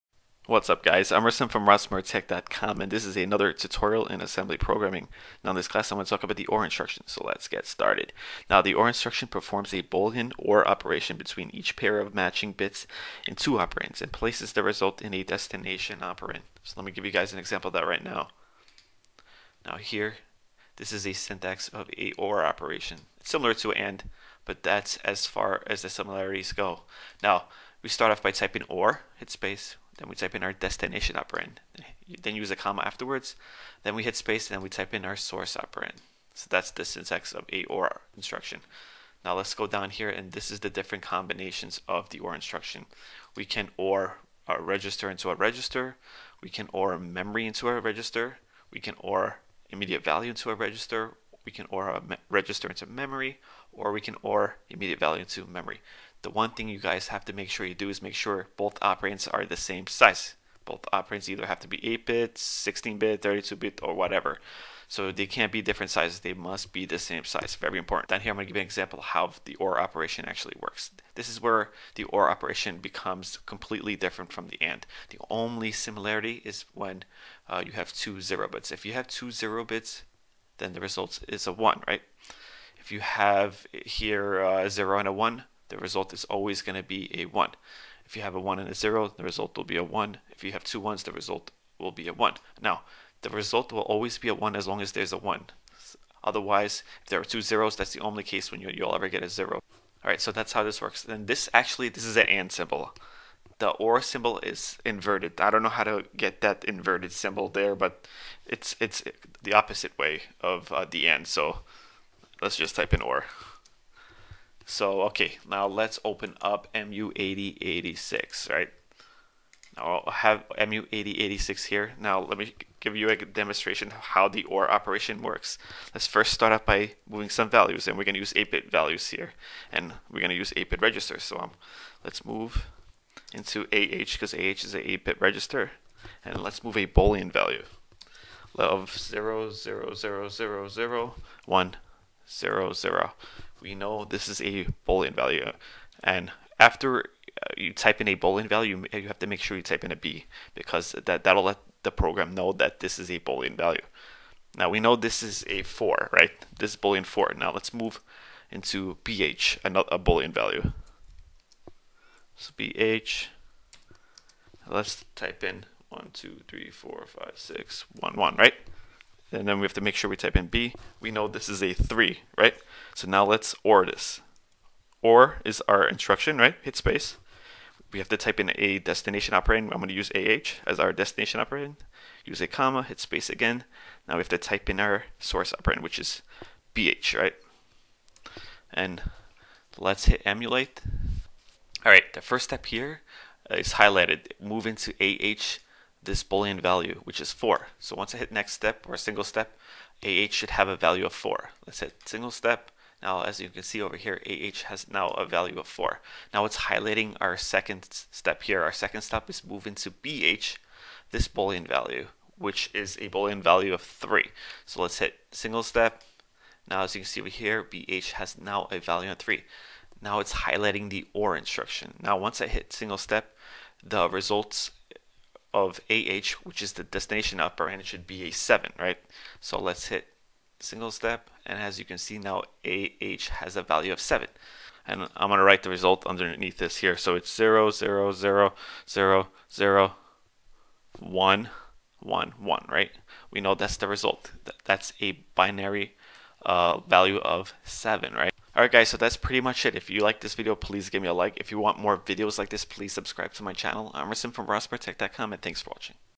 Assembly Language Programming Tutorial - 35 - Or Instruction
Rasim from Rasmurtech.com give us another tutorial on Assembly Language Programming . In this tutorial Rasim shows us how to use the OR Instruction.